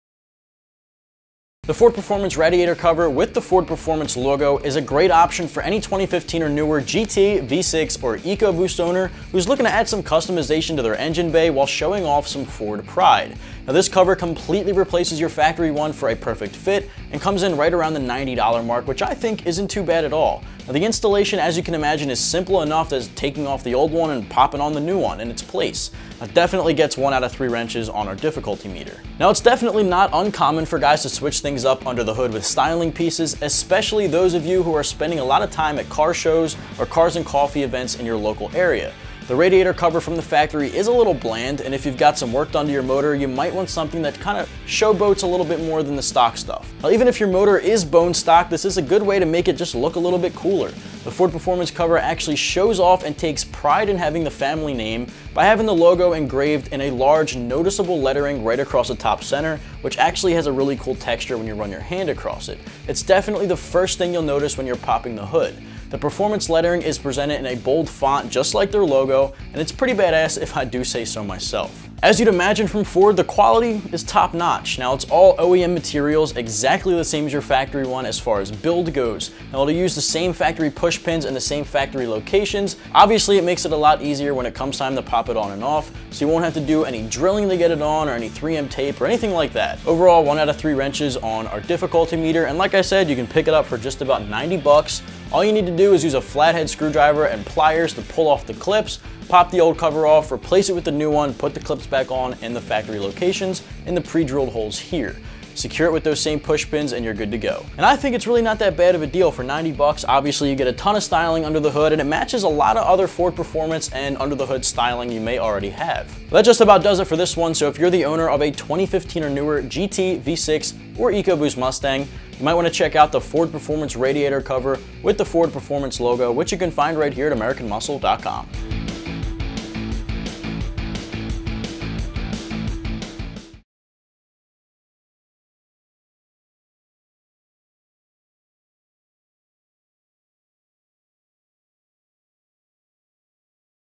2015-2017 Mustang GT, EcoBoost, V6 Ford Performance Radiator Cover Review & Install
Add some under hood style to your Mustang with this Ford Performance Radiator Cover that features the Ford Performance logo! This radiator cover is made from high-quality materials that match the performance and style already on your Mustang.

Item 397596
MPN# M-8291-FP

Sleek, Custom Engine Bay. Looking for a genuine part to customize the engine bay in your 2015 to 2017 Mustang GT, V6, and EcoBoost? Look no further than the Ford Performance Radiator Cover. Bridging the gap between the taillights to cover the radiator and streamline the engine bay, this piece is a genuine Ford replacement part and is extremely easy to install. The radiator cover is one of the first things you will see as you raise the hood and this Ford Performance Radiator Cover has ‘Ford Performance’ prominently written across the top to show off your true blue Ford pride!

Proudly made in the USA. The Ford Performance Radiator Cover is proudly made in the USA. Designed using original CAD specifications from the factory, the radiator cover is designed to fit cleanly in the factory location with no cutting, no drilling, and no adhesives required. This kit is designed to reuse the factory hardware for a simple and straightforward installation.

Simple to Install. You can finish the job in approximately 10 minutes or less using only a flathead screwdriver and a pair of pliers. The first step is to unplug the factory push pins to remove the OEM radiator cover. After that, simply place the Ford Performance Radiator Cover in the factory location and secure the cover by inserting the push pins. Close the hood and you’re ready to show off your Ford pride!